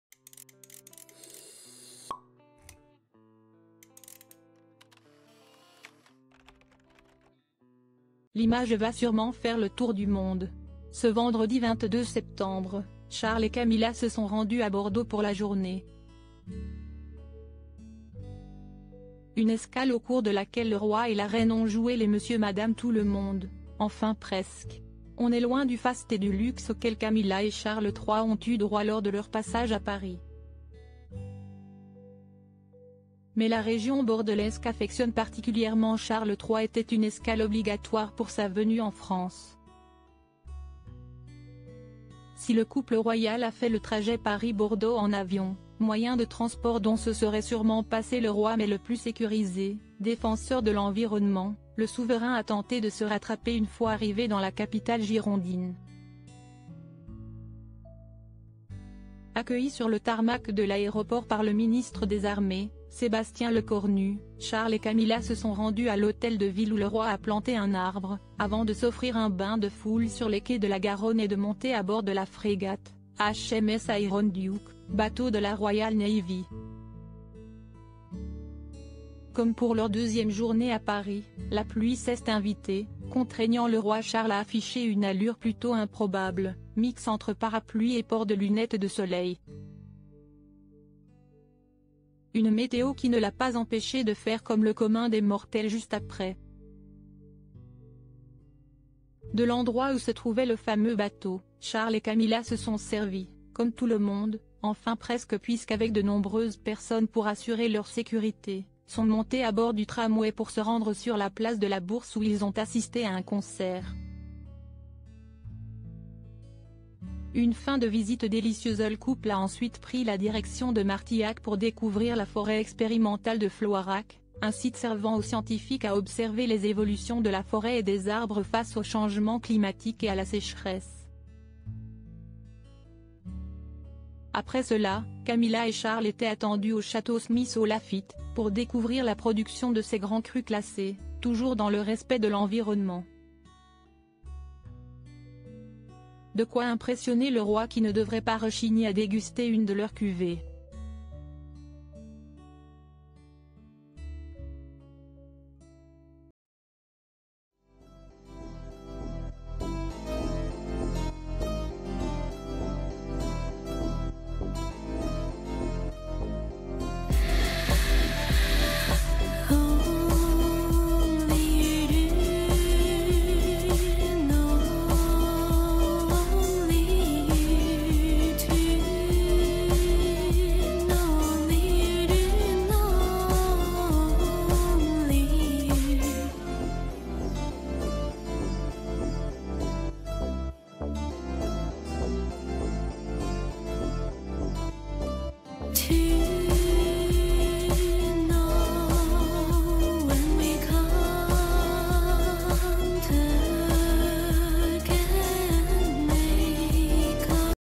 PHOTOS Charles III et Camilla à Bordeaux, monsieur et madame Tout le monde un couple presque lam
PHOTOS Charles III et Camilla à Bordeaux, monsieur et madame Tout-le-monde : un couple (presque) lambda dans le tram !